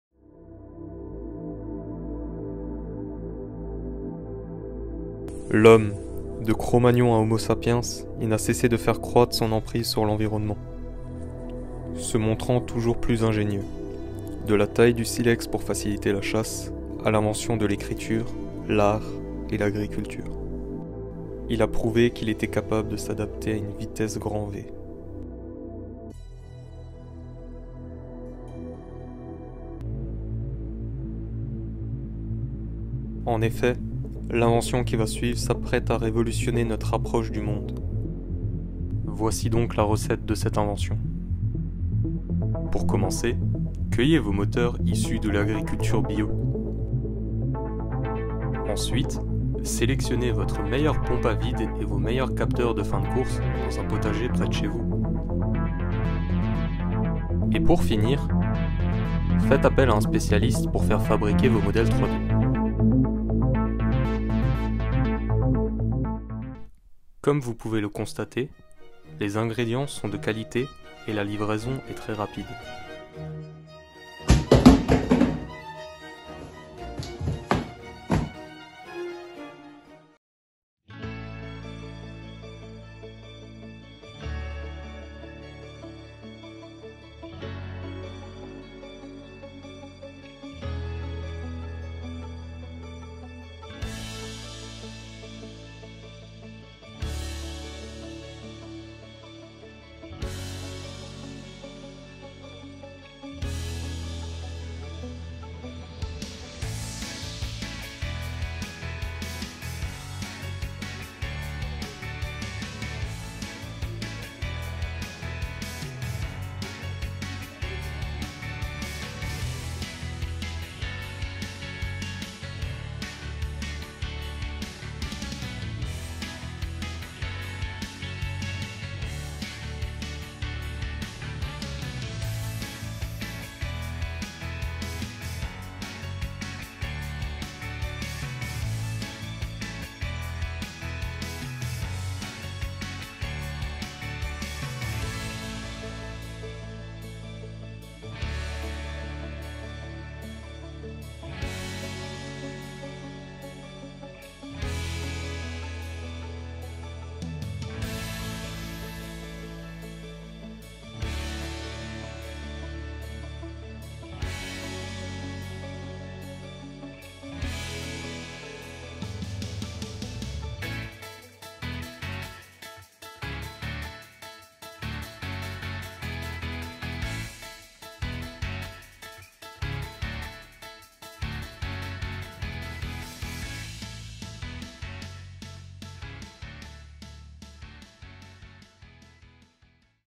M&M's-Sortiermaschine - Projektarbeit 2021
Zusammenfassung der Projektarbeit 2021 - Entwicklung einer Sortiermaschine
FTM Promotion DESCARTES - Thomas P., Gil F--T.

Hochschulstudium Mechatronik Trinational, verteilt auf die drei Länder F, D und CH.